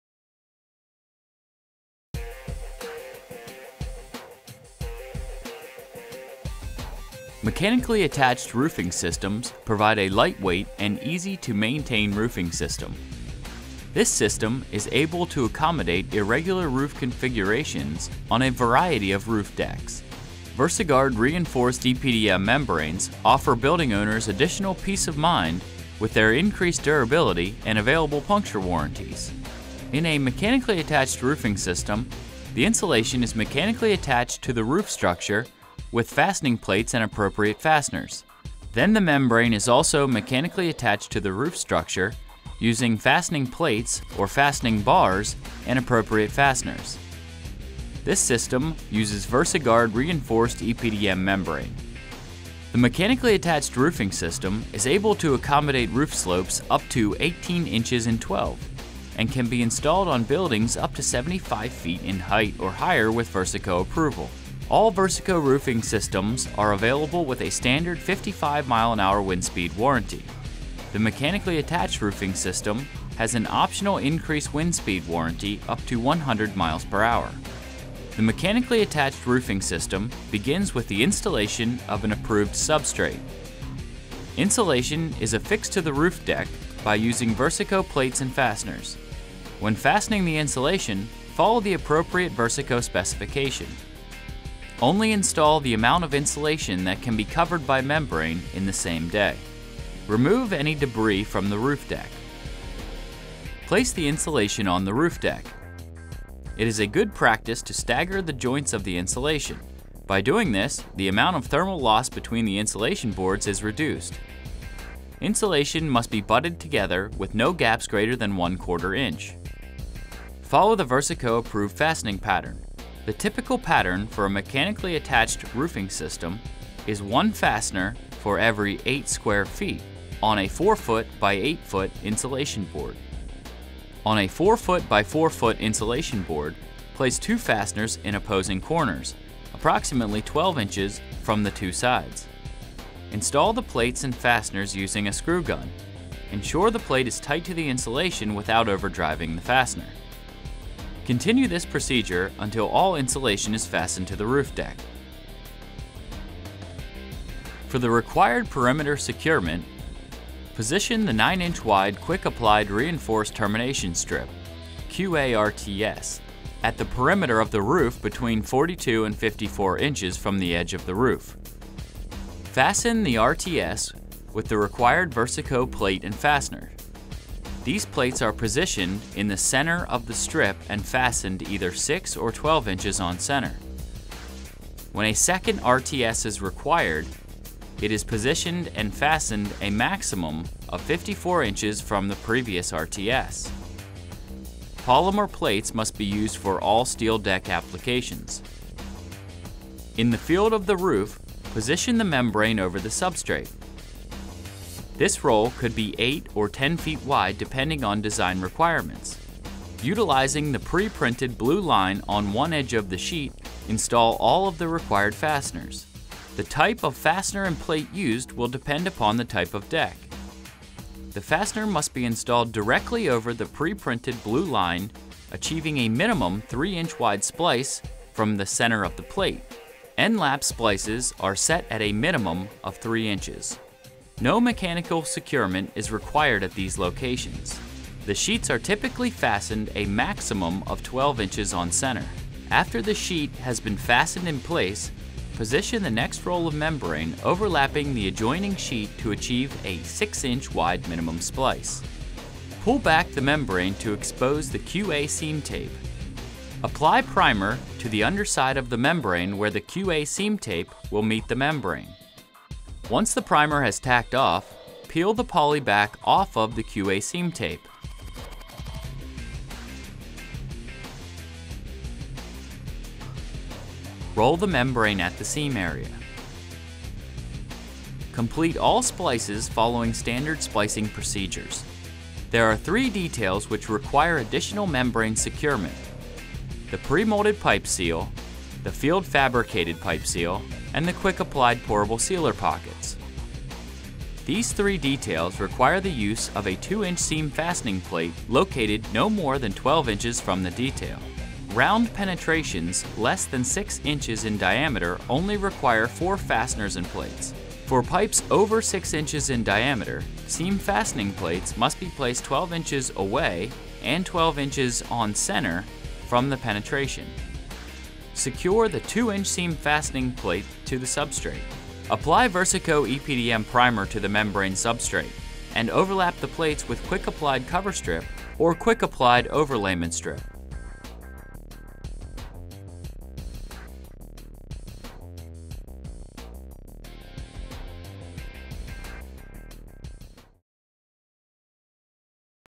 VersiGard EPDM Mechanically Attached System Installation
Video explaining Versico's VersiGard EPDM Mechanically Attached System Installation